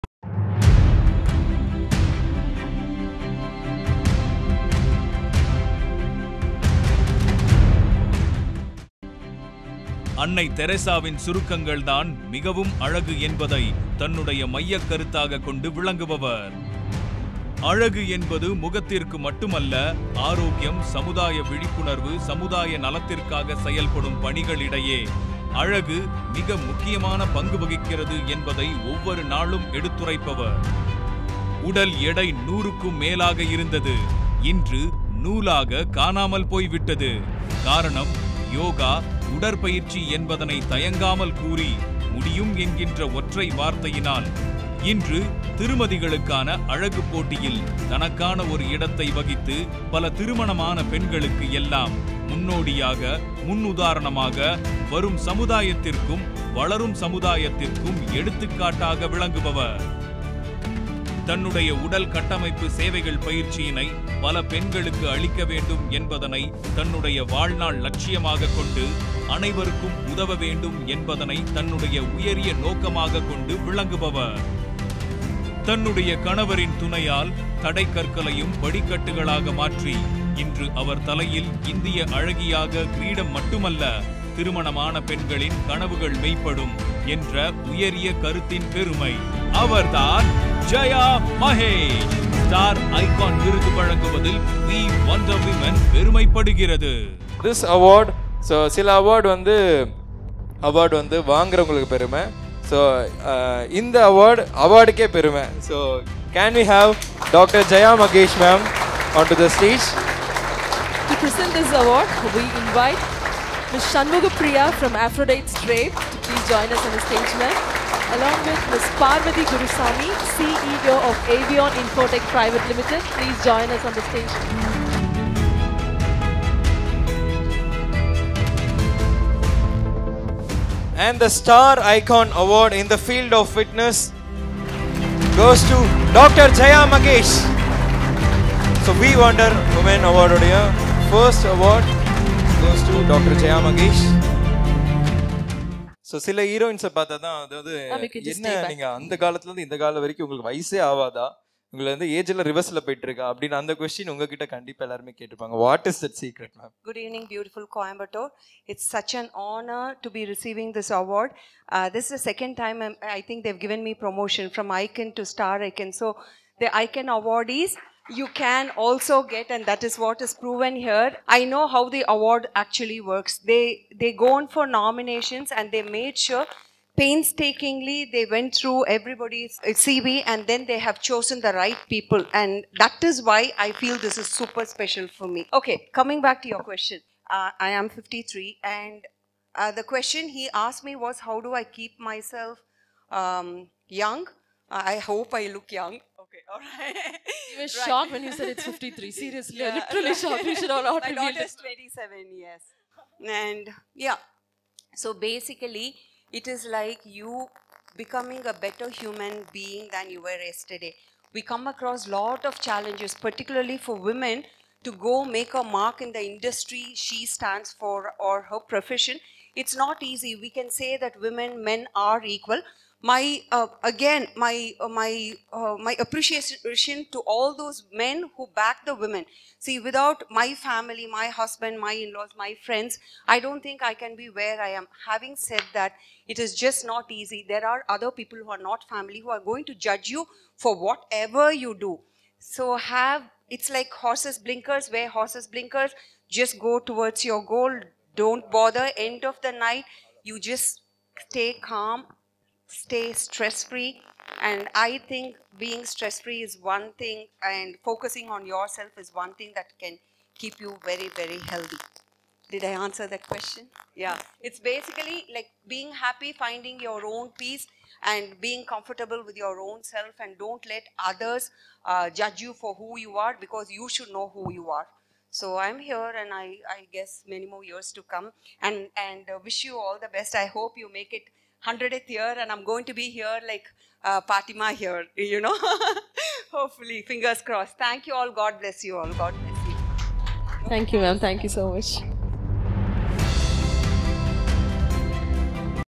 Dr. JayaMahesh - SunTv Fitness Guru - Star iCON - We Wonder Women Awards 2022
Jayashree Chandramohan popularly known as Dr. Jaya Mahesh is an Indian pageant winner, model and fitness therapist. She was a homemaker before overcoming her post-pregnancy health issues and starting her career.
Jaya Mahesh was suffering from Post Pregnancy trauma and abnormal weight gain.She gained over 118 kilograms (260 lb) after her pregnancy. She also lost her eyesight due to her abnormal weight gain.She overcame her personal struggles with support from her husband and children - as she quotes,she framed her own routines of food and exercise habits.
Jaya Mahesh had her first big success and recognition when she was crowned Mrs. Coimbatore in the year 2006. She went on to win and crowned as Mrs. India Earth Classic in the year 2016.Jaya Mahesh was recently adjudged the third runner up at Mrs Globe Classic event in California. She also bagged the Mrs. Photogenic crown.

1. NEW ERA WOMEN OF CHANGE - Kamala Power Women Award by Shri. Bhagat Singh Koshyari (Governor of Maharashtra) at Raj Bhavan , Mumbai on March 8th 2022.

2. The Honorary Doctorate Award is a symbol of tribute and honor and Jaya Mahesh is the recipient of Honorary Doctorate (D.Litt.) Awards by University of South America.

3. The second Honorary Doctorate in FITNESS & EXERCISE SCIENCE by the Global Human Peace University in March 2022.

4. Mrs. India Earth Classic 2016 - Mrs. Jayashree Mahesh is the first lady from South India to win this coveted title.

5.Jaya Mahesh represented our country in an international pageant for women above 45 in Mrs. Globe Classic, which happened from June 13th – June 18th , 2018 at Palm Springs, California, US and was crowned as the Runner up. She also won Mrs. Photogenic and Mrs. Indian Ocean title.

6. Jaya Mahesh has been given Outstanding Achievement Award by Rotary International District 3201 in recognition of her outstanding Achievements in Social Service.

7. “ WOMEN ICON AWARD 2020 “ – handed over by the Hon’ble Sanyuktha Bhatia Ji – Mayor, Lucknow city at Renaissance Lucknow on 17th Nov 2019

8. VETERANS 1st NATIONAL WOMEN MASTERS ATHLETIC CHAMPIONSHIP 2019 conducted by MASTERS ATHLETICS FEDERATION OF INDIA and won a BRONZE MEDAL in discuss for TAMILNADU

9. Vanakkam Thamizha in Sun TV

10. Pride of Namma Chennai 2019 by EWC Iconic Women Summit

11. Co-Founder USTATI – RISE ABOVE: An initiative to render services however big or small for the under privileged. USTATI has always been committed to worthy causes and been instrumental in bringing about change in the lives of the underprivileged.